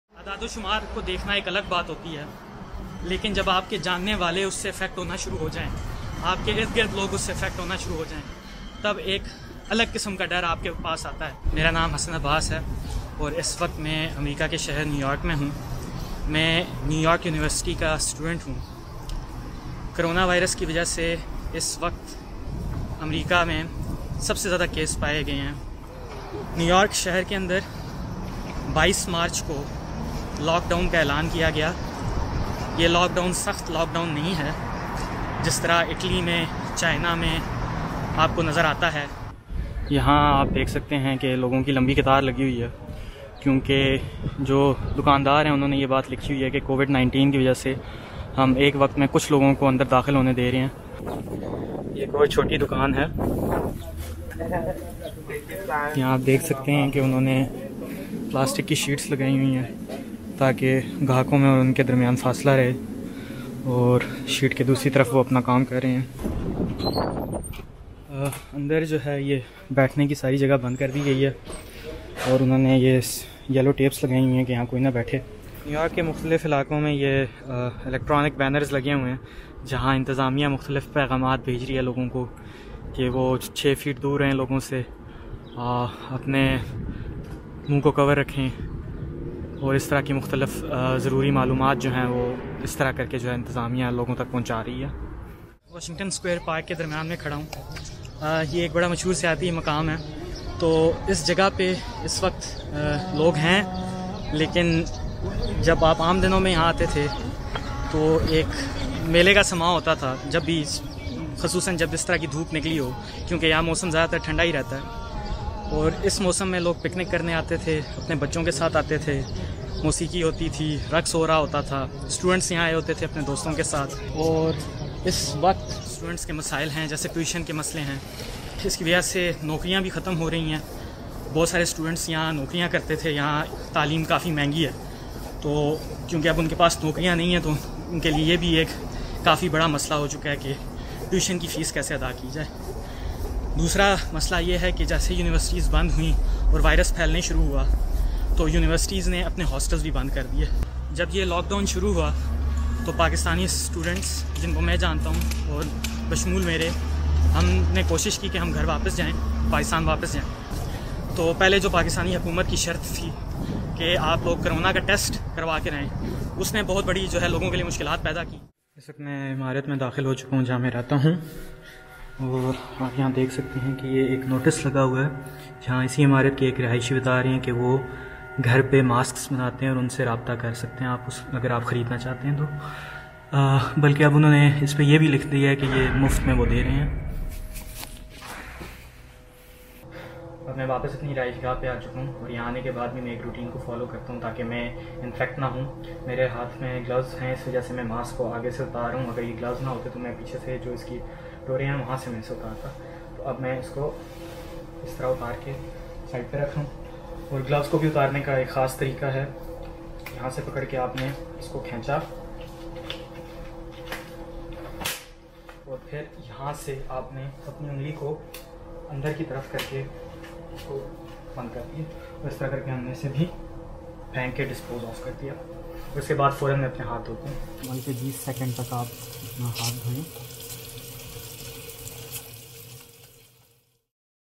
Pakistani student talks about lock down in New York City - BBC URDU
امریکہ کی ریاست نیویارک میں کورونا وائرس کے باعث دنیا میں متاثرہ ممالک سے بھی زیادہ ہلاکتیں ہوئی ہیں۔
لاک ڈاؤن میں شہر کے حالات اور معمولاتِ زندگی کے بارے میں جانیے نیویارک سٹی میں مقیم ایک پاکستانی طالبعلم سے۔۔۔

ویڈیو: حسن عباس، محمد نبیل